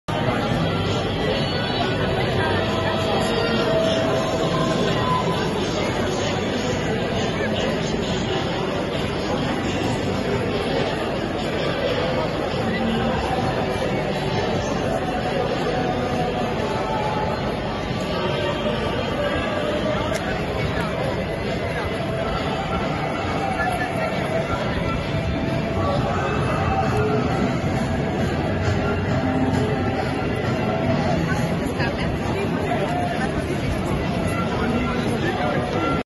quatrième journée de mobilisation contre la réforme des retraites
Paris, le 11 févr. 2023
A Paris, 112.000 manifestants ont défilé de République à Nation